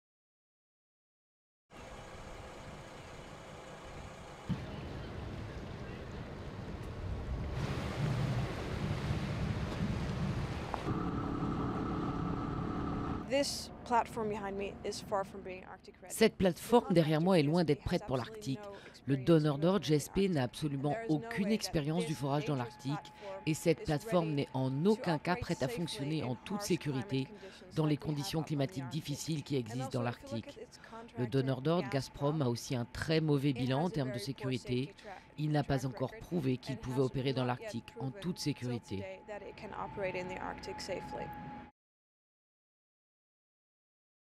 Pays-Bas: des militants de Greenpeace arrêtés
La police néerlandaise a arrêté mardi 27 mai six militants de Greenpeace alors qu'ils menaient une action pour protester contre l'exploitation d'hydrocarbures dans l'Arctique. Durée: 00:44